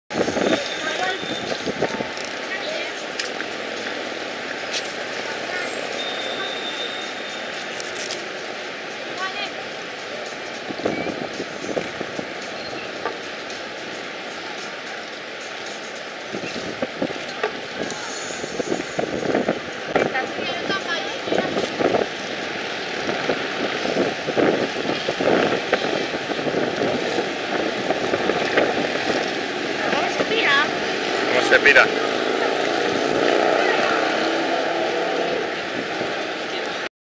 Prueba Toshiba P30
Vista de Montjuich (Barcelona) desde el centro comercial Las Arenas.